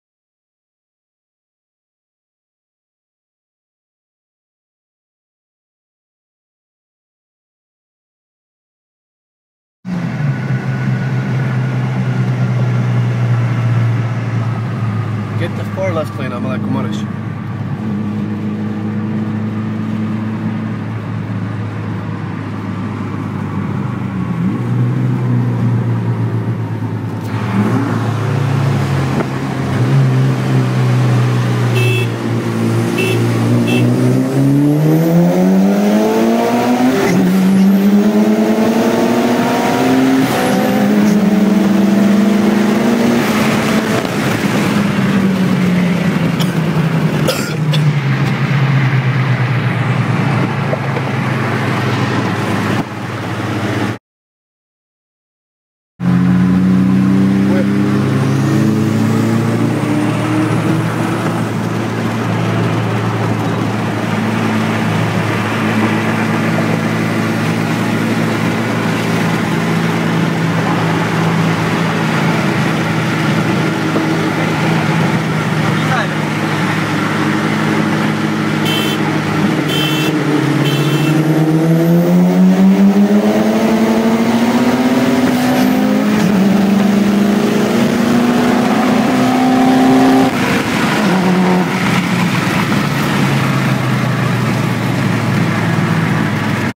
Evo8 Vs FP Green Evo9
My friend and I decide to mess around a little after The Friday Night Lights Meet organized by JDM Chicago.

I'm in the Evo 8(Camera Car) which has a Evo9 turbo and the Evo IX has FP Green